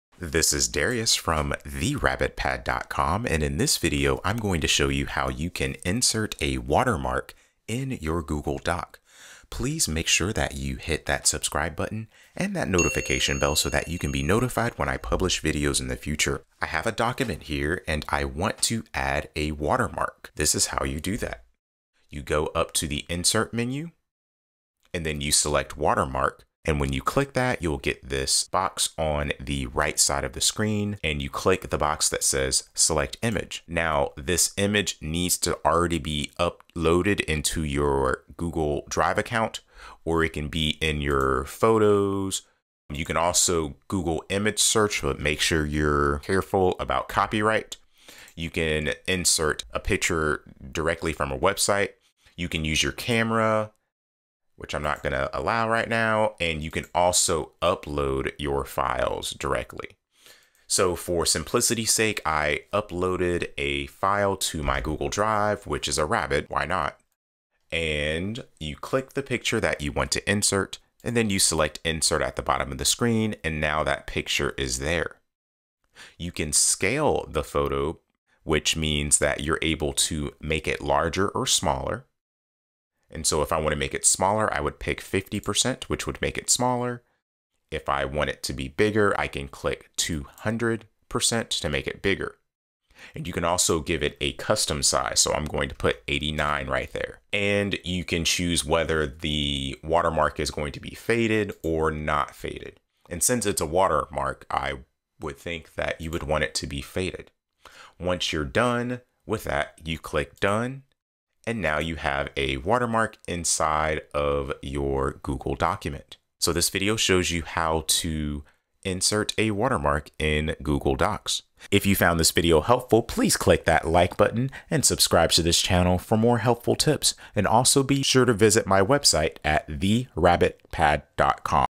How to Use a Watermark in Google Docs
This video will show you how you can use watermarks in Google Docs.